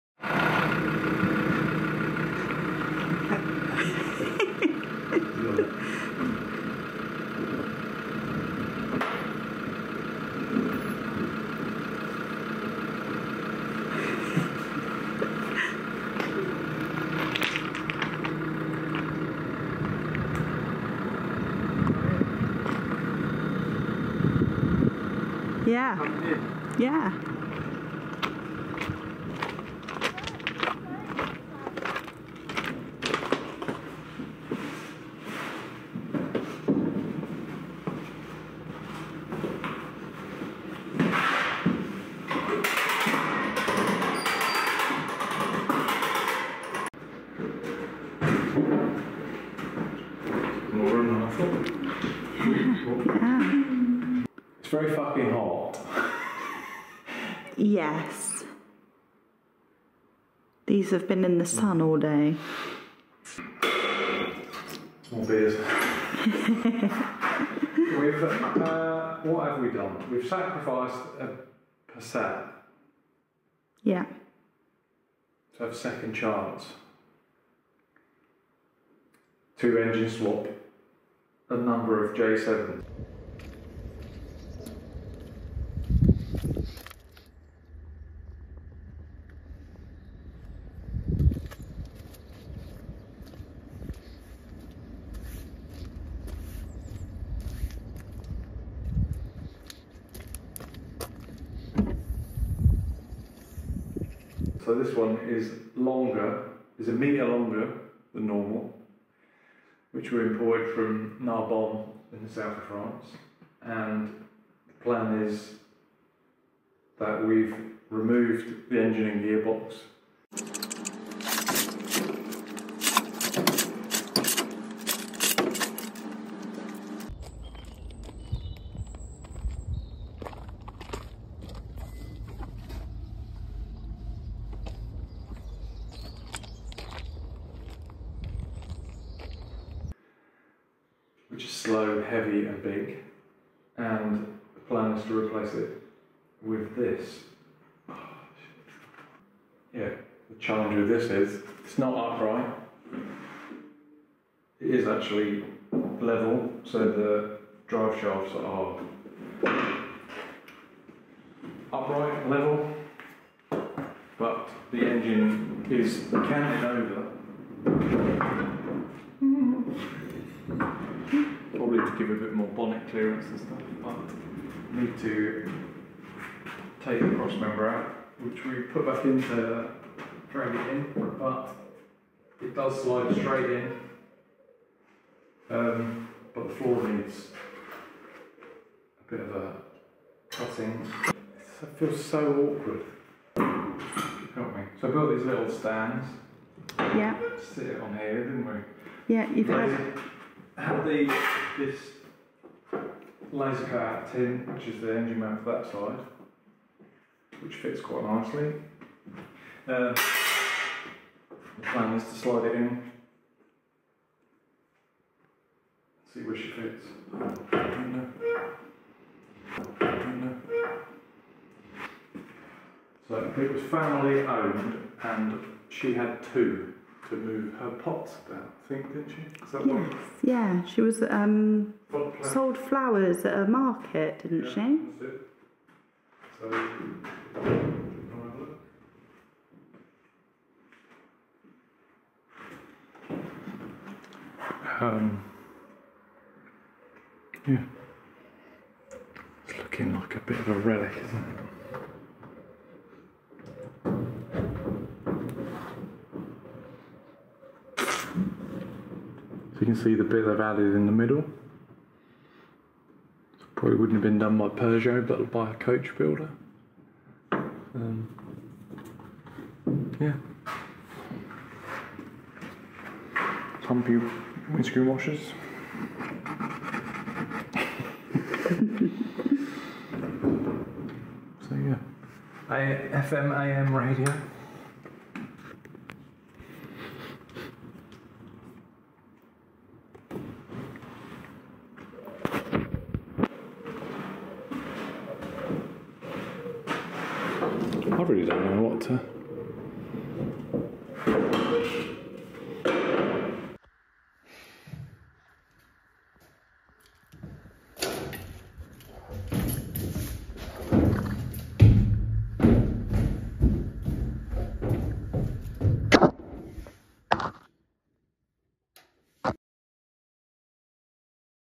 Peugeot J7 VW Passat Engine Conversion
We are engine swapping this LWB J7 we imported from Narbonne Southern France.